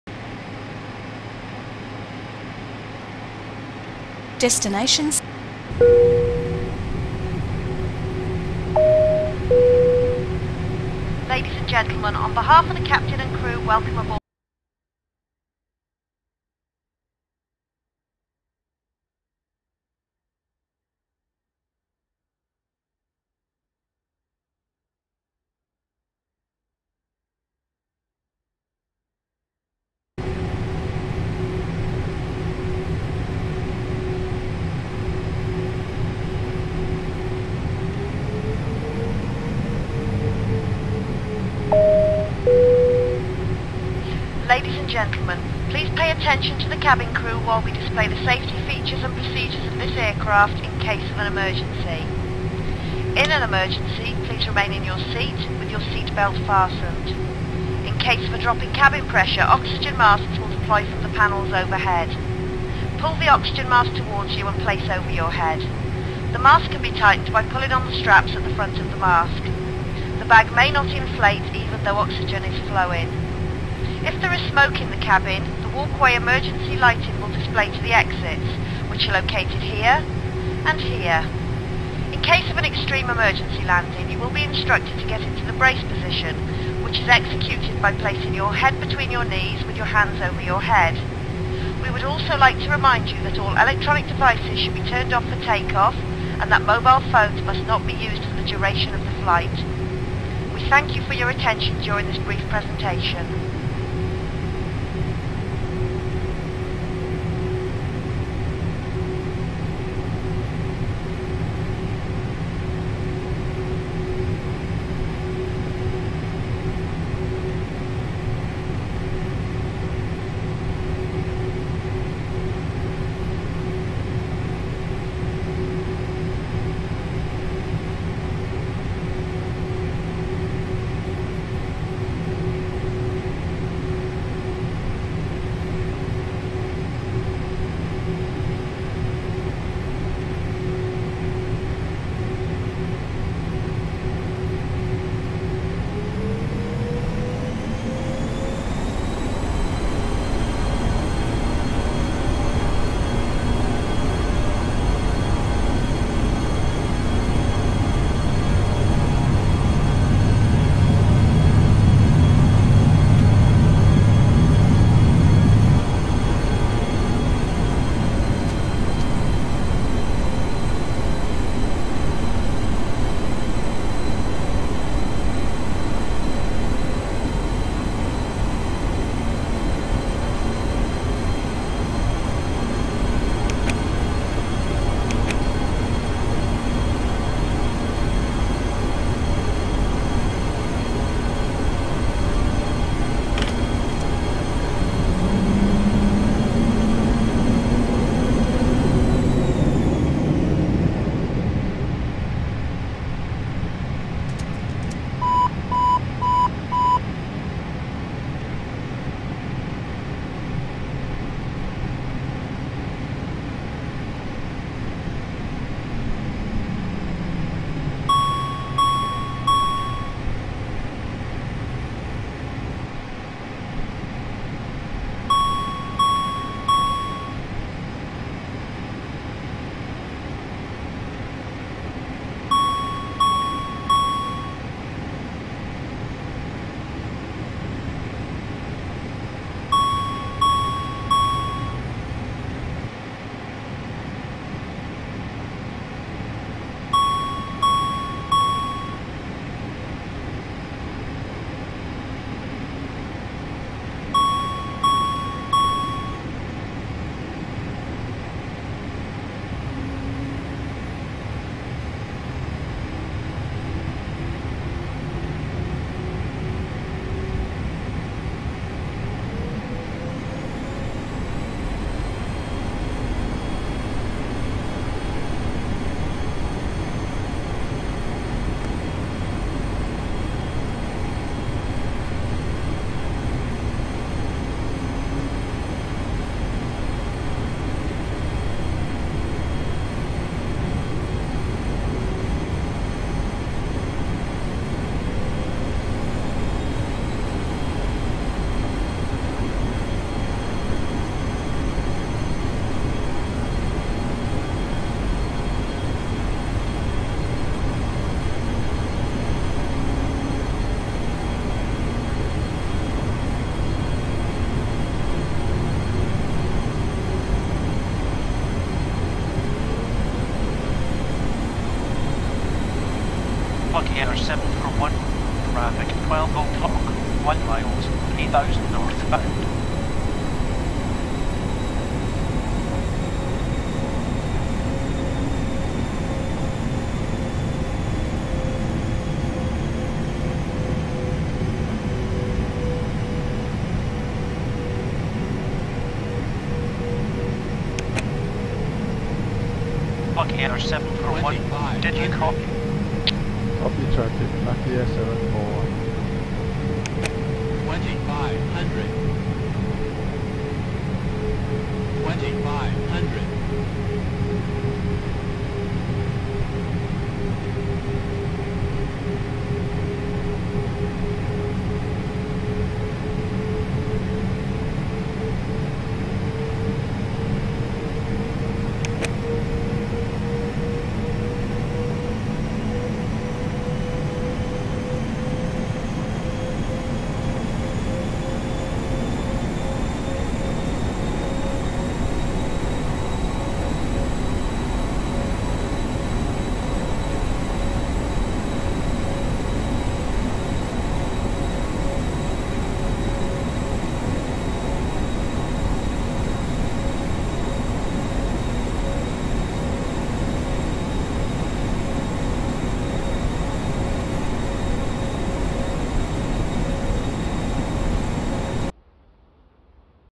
Engine shutdown and near miss!!!! Bad things happened together!!
I suffered a 4 engines shutdown right after takeoff at NTMN, just as I was reviving the engine one by one, a Boeing 737 suddenly appeared at 12 O'clock coming straight at me. Fortunately he is a little lower as I was not in the power to evade at all having only started 2 engines. Call this the epic incidents day for me at the South Pacific...